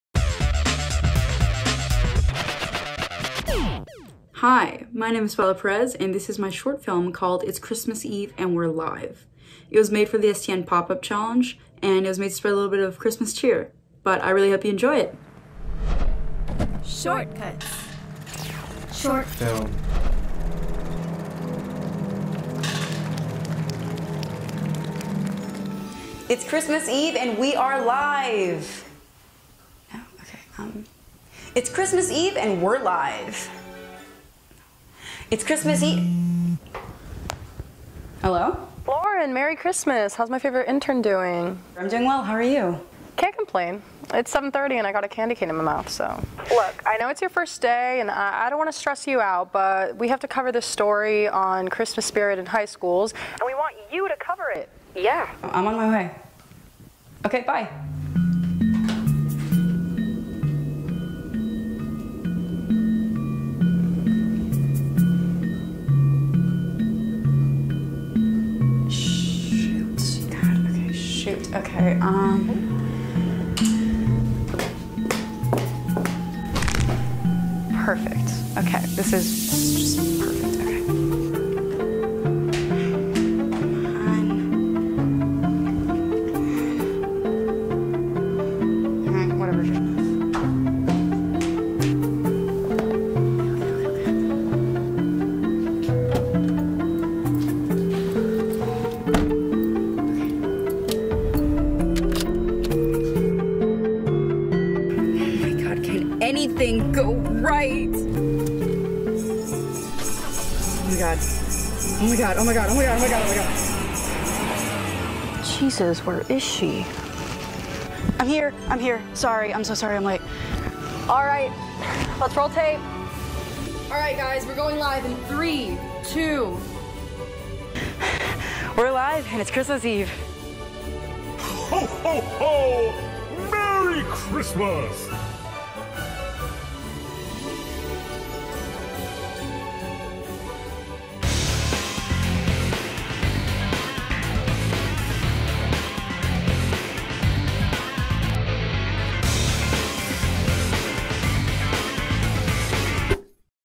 It's Christmas Eve, and We're Live! - Coral Reef (Reef Film Club)
Amid the hectic pathway to work, Lauren Prescott faces her first day as a News Anchor and hopes that the Christmas Spirit might lend her a hand.

This short film was created by Paola Perez from Coral Reef High.  With Carolina Calleiro, Lisa Maarek, Matias Cabellio Del-Boccio  3/24/22